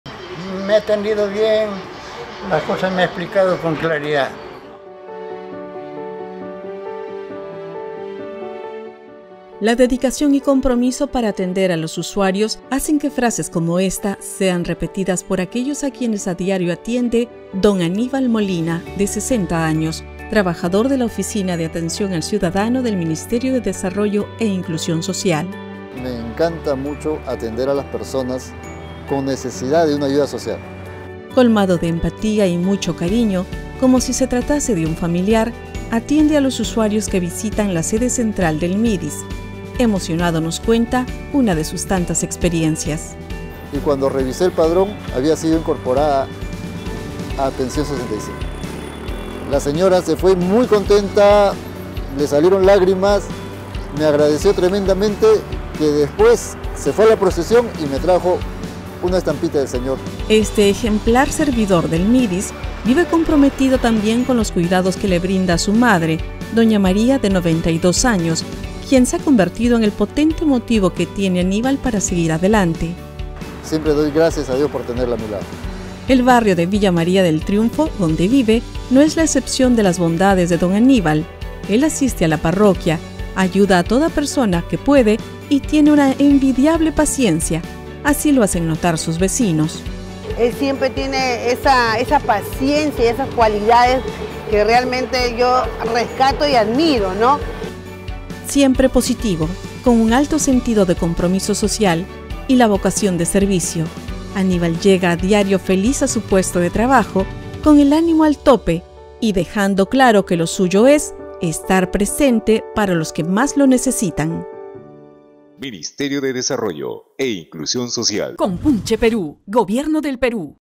HistoriasQueNosUnen | Conoce a Aníbal Molina, trabajador del Midis, quien atiende a los ciudadanos.
HistoriasQueNosUnen | Conoce la historia de Aníbal Molina de 60 años, trabajador de la Oficina de Atención al Ciudadano del Midis, quien atiende a los usuarios y a los ciudadanos en general con un alto sentido de compromiso social y vocación de servicio. 🇵🇪